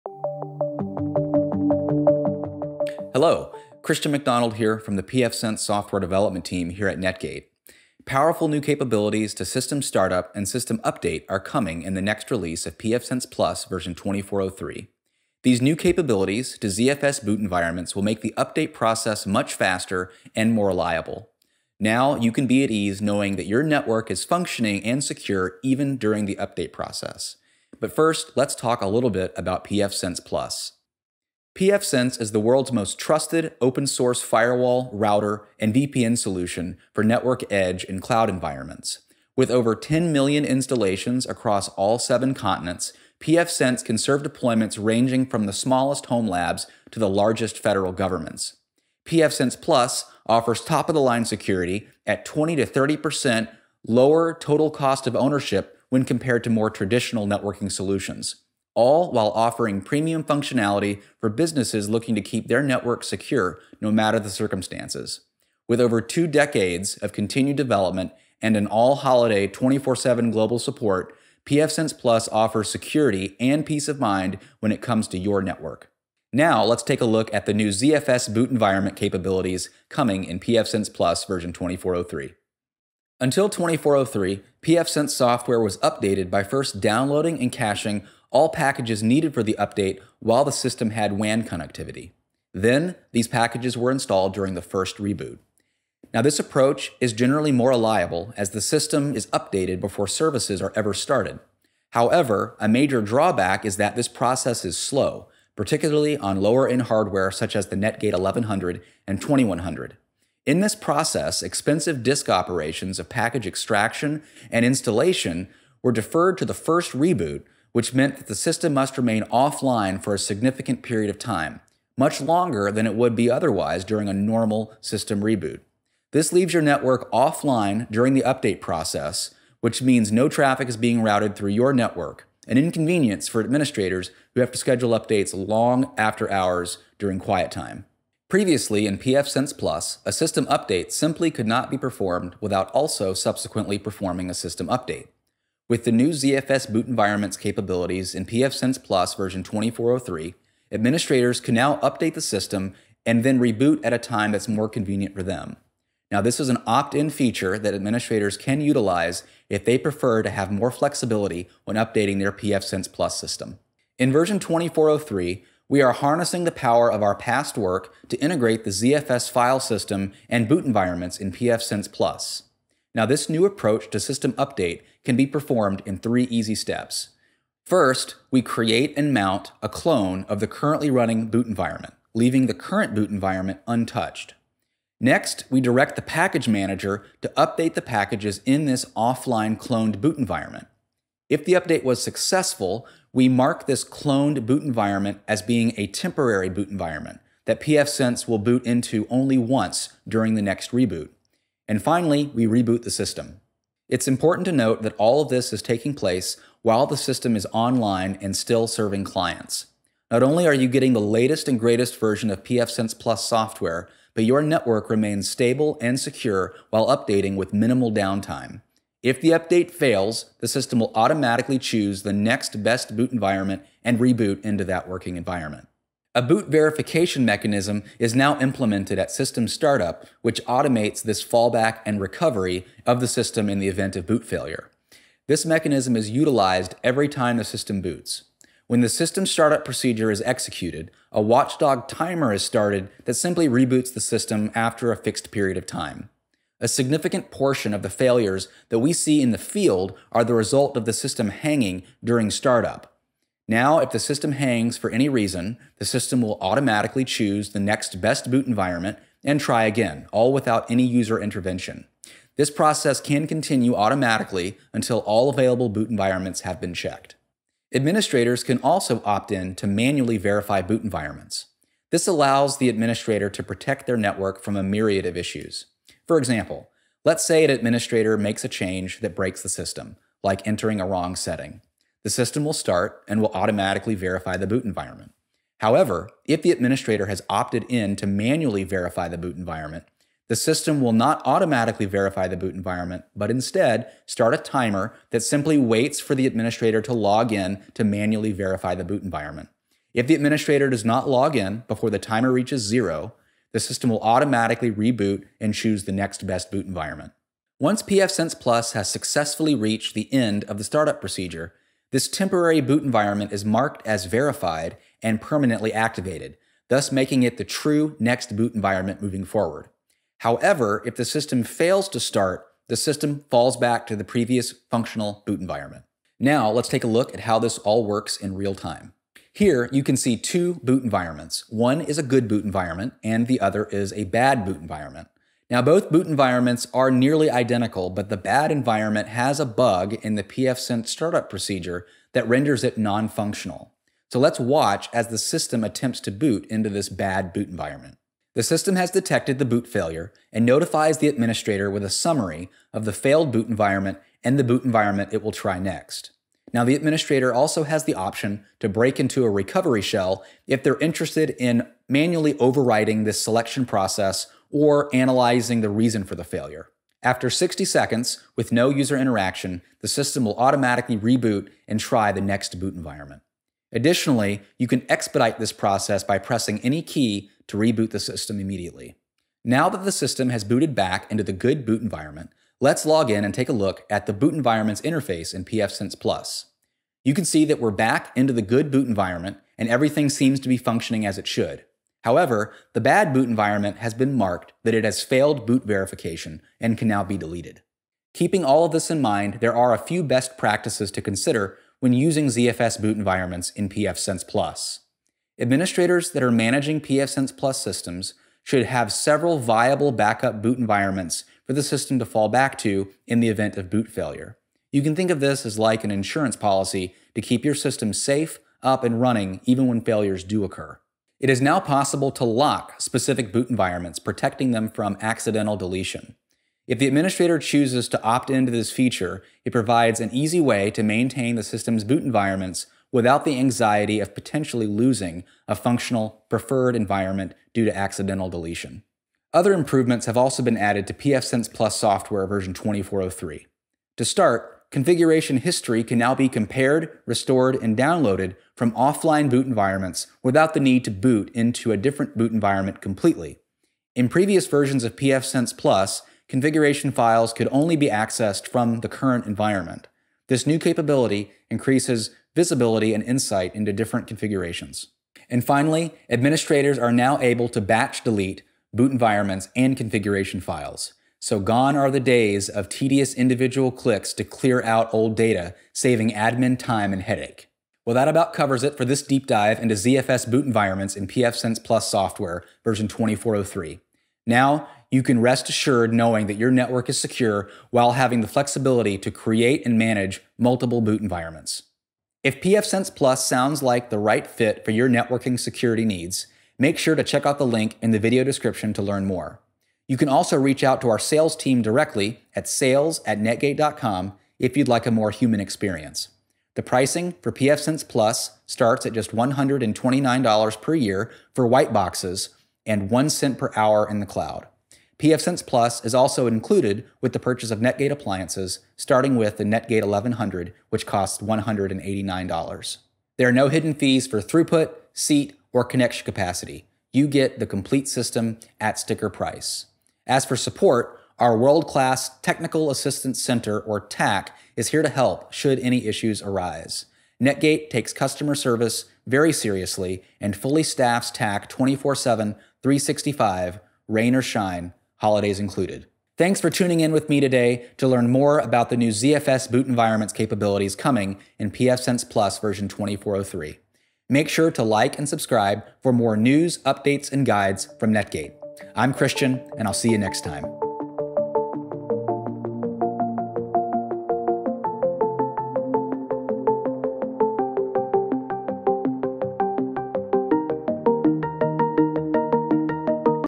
Deep Dive into the NEW ZFS Boot Environments feature in pfSense Plus v24.03!
Take a Deep Dive into the NEW ZFS Boot Environments feature coming to pfSense Plus Software v24.03! Discover a new approach to system updates with Christian from the pfSense development team.